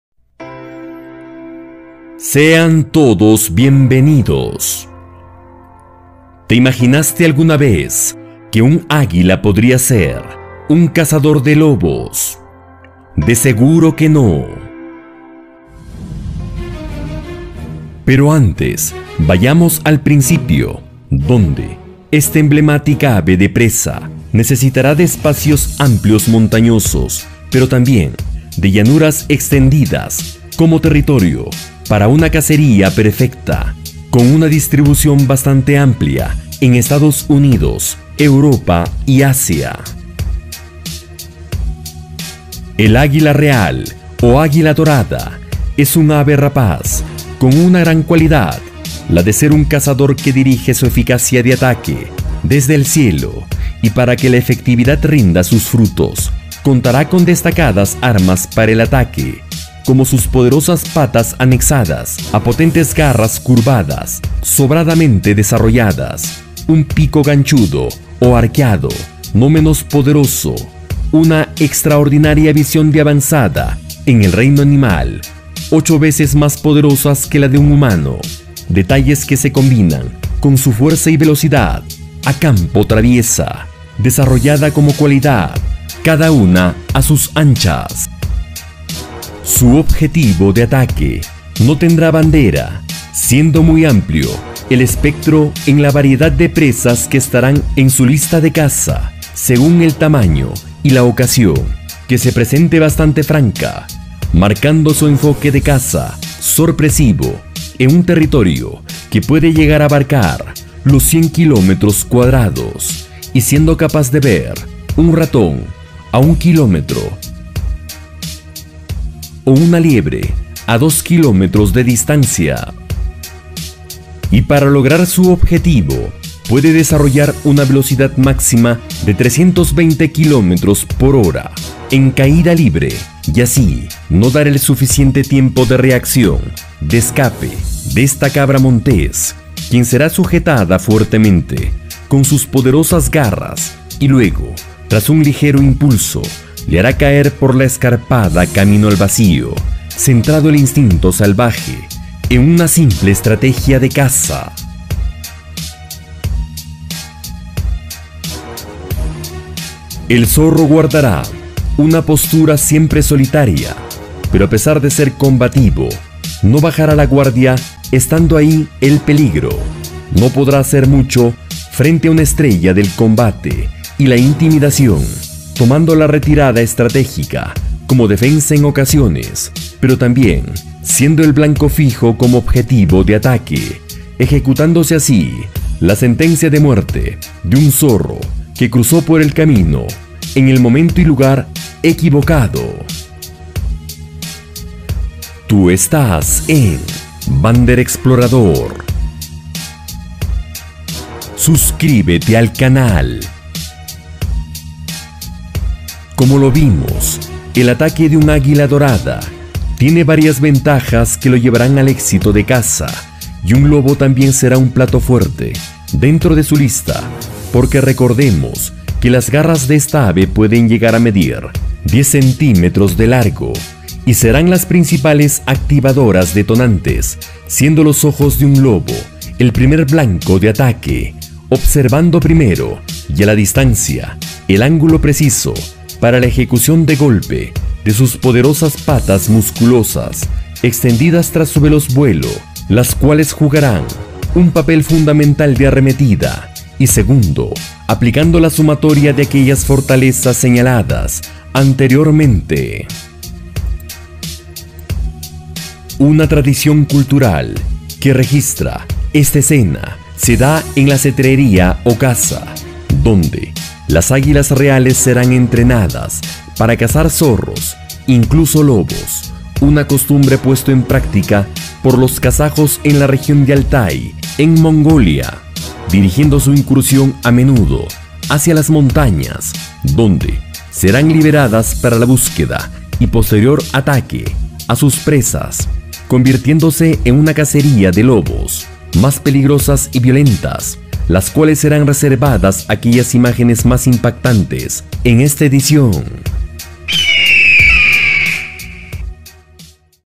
Esto Es Lo Que Hace Un Águila antes de Cazar Un lobo
El águila real o águila dorada cuenta con varias armaduras que lo convierte en el verdadero terror de lobos, zorros, corzos, liebres, incluso ratones, un abanico muy amplio de potenciales presas, que serán acechadas bajo el factor sorpresa de ataque, detalles que lo conocerás en esta edición, sorprendiéndonos a todos la información de principio a fin.